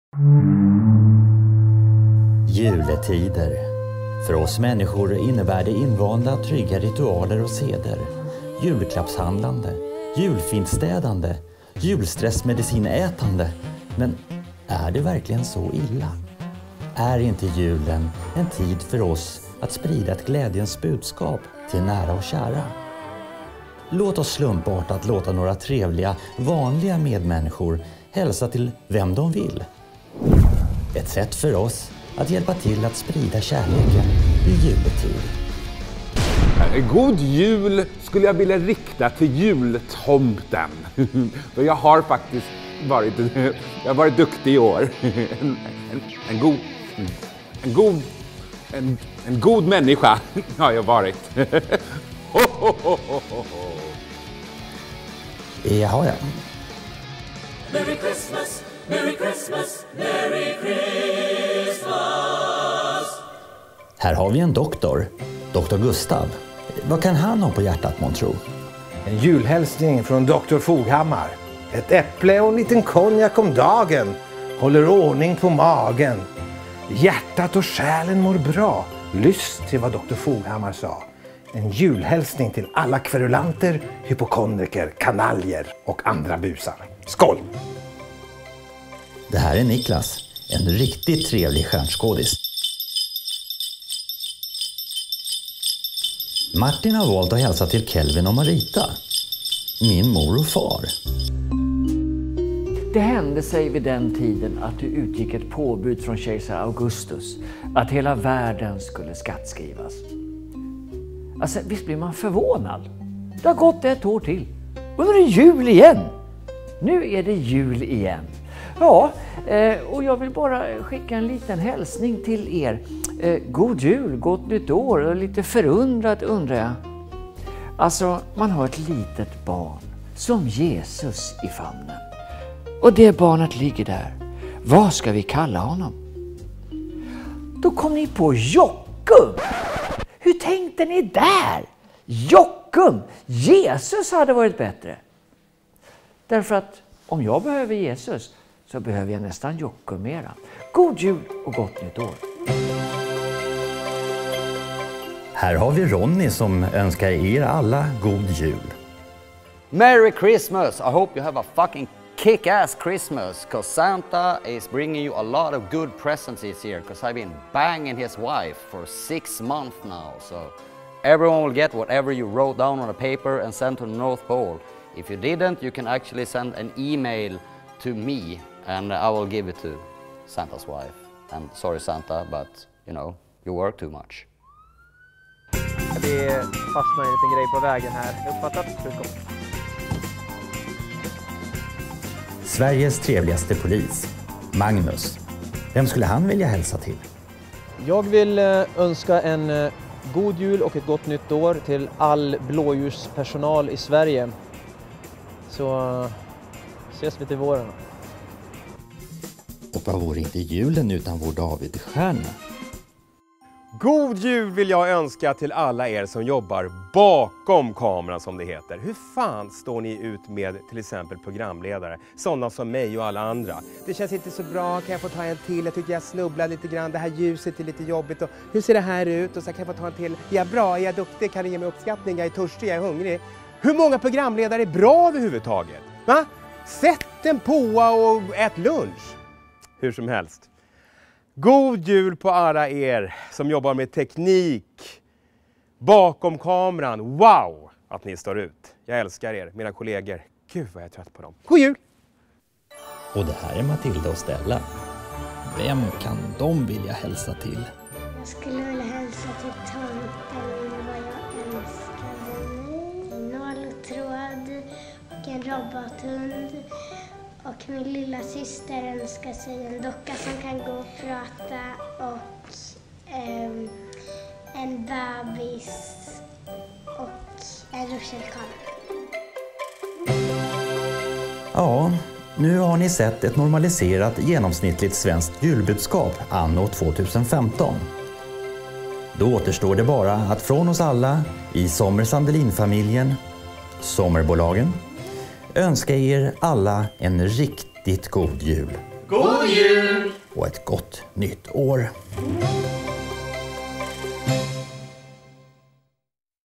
ÅRETS JULFILM 2015 SOMMERBOLAGEN - David Helenius, Martin Timell och en massa andra julhälsningar
I år så upplåter vi julfilmen åt Svenska folkets kärlekshälsningar i dessa nu så kallade juletider.. David, Martin, Nalle, Niklas, Magnus, Ronnie, Gustav, Matilda och Stella bjuder på ett fullständigt, genomsnittligt svenskt julbudskap - Anno 2015. ..Ja och så vi förstås - Jennie, Jokum, Sól, Linnéa, Alexandra och katterna Nemo och Dorian.